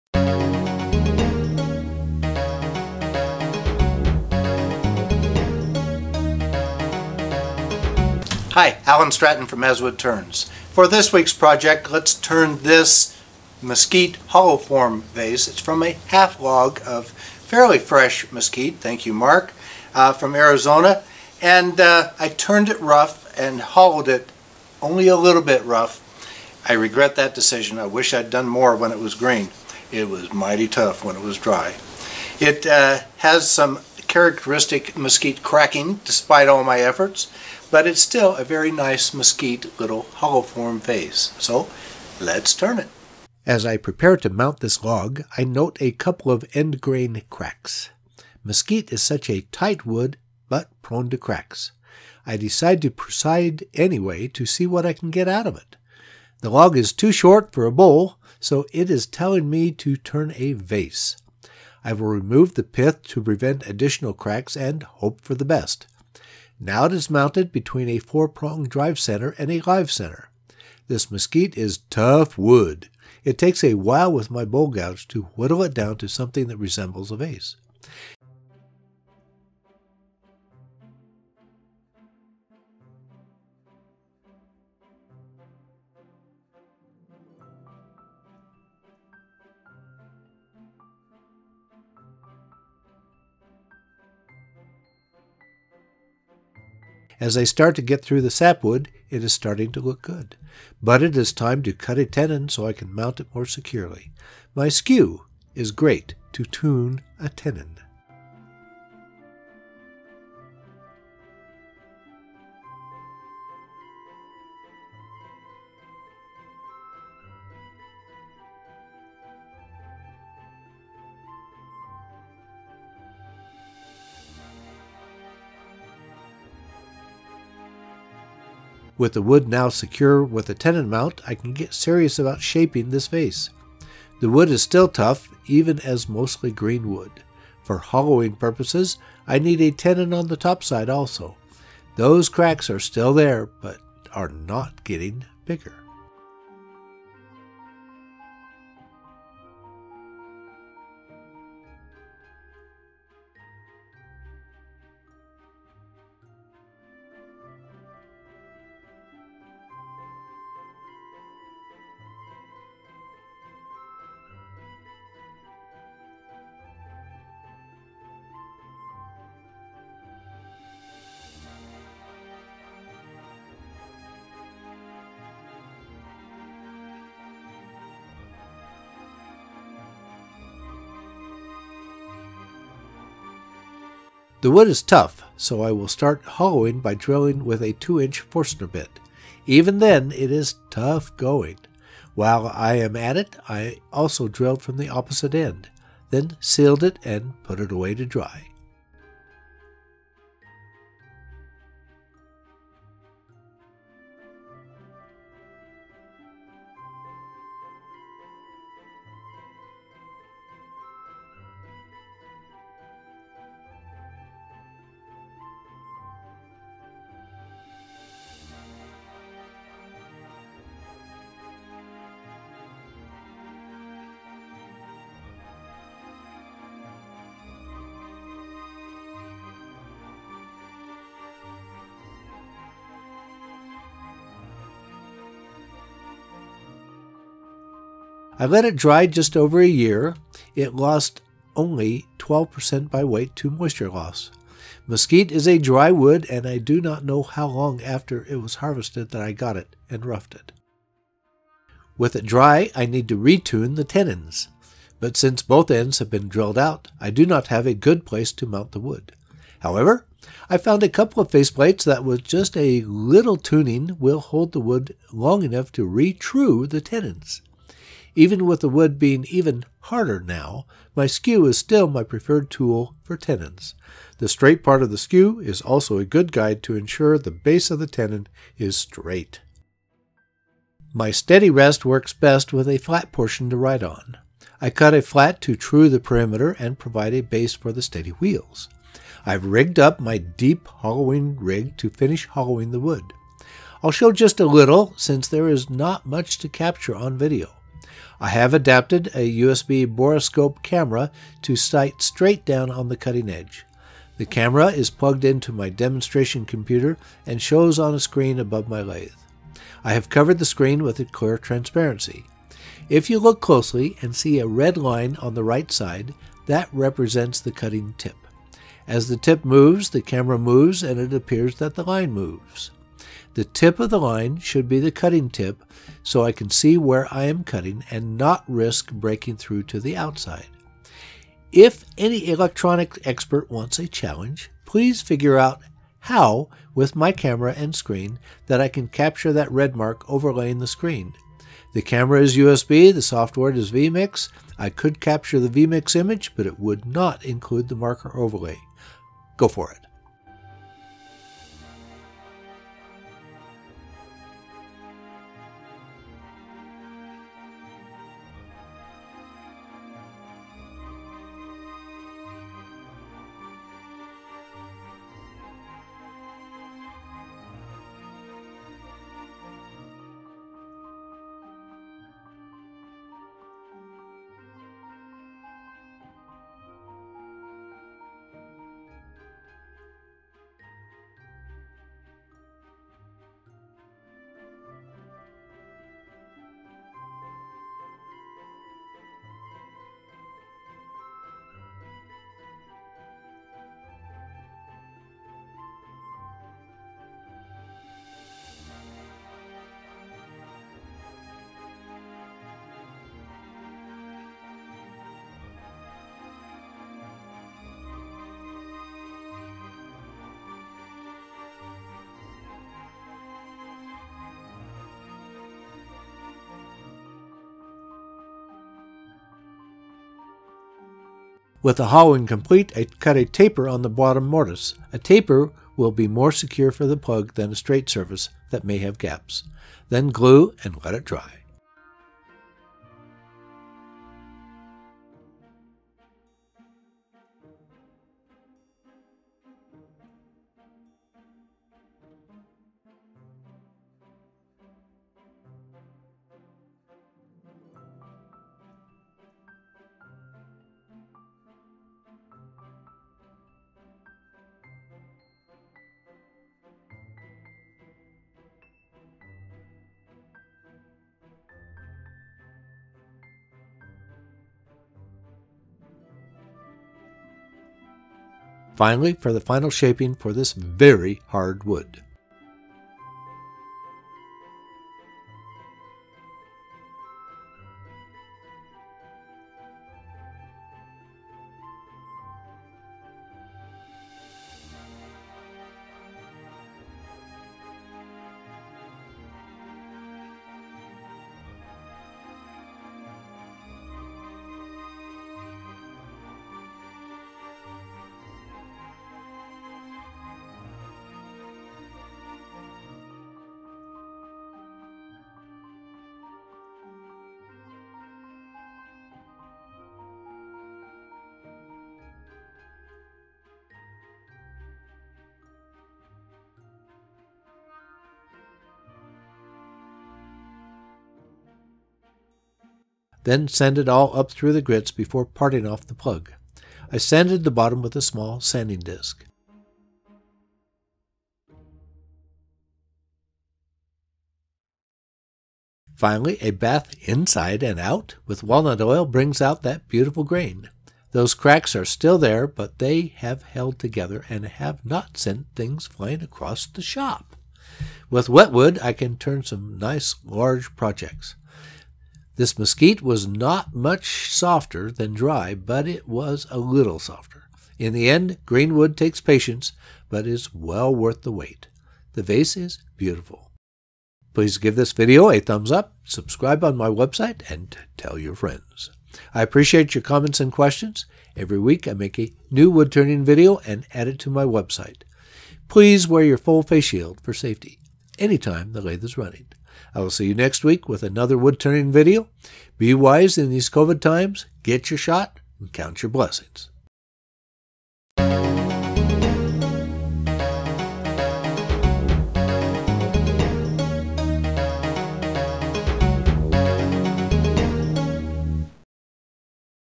Woodturning Mesquite Vase - Rough and Tough From Green To Finished
I picked up this mesquite in Arizona over a year ago. Shortly after bringing it home, I rough turned and partially hollowed it. I only partially hollowed it because I was still working on my hollowing rig at the time. I regret not hollowing it more while green and a little softer.

I sealed the wet wood with Bowl Save from Craft Supplies USA, a PVA based wood sealer and let it dry.

Once dry and harder, I finished hollowing and then finished the outside. It is difficult to show hollowing since the real action is inside the vase - out of sight. I use a USB borescope camera on the hollowing rig. The camera moves with the cutting bit. Even though the bit is inside the vase, it still shows the relative position of the cutter and the outer wall of the vase on the screen.

For those wondering why the hollowing video segment is so poor. The tool is marked on an transparency covering the screen. What you can see is a camera shot of my small screen including the transparency. For you tech savvy people, I challenge you to find a better way to show this setup.

The vase is five inches diameter and six inches tall finished with walnut oil.

For the steady rest, please see my video where I review it.
Woodturning - My DIY Steady Rest Is Still Steady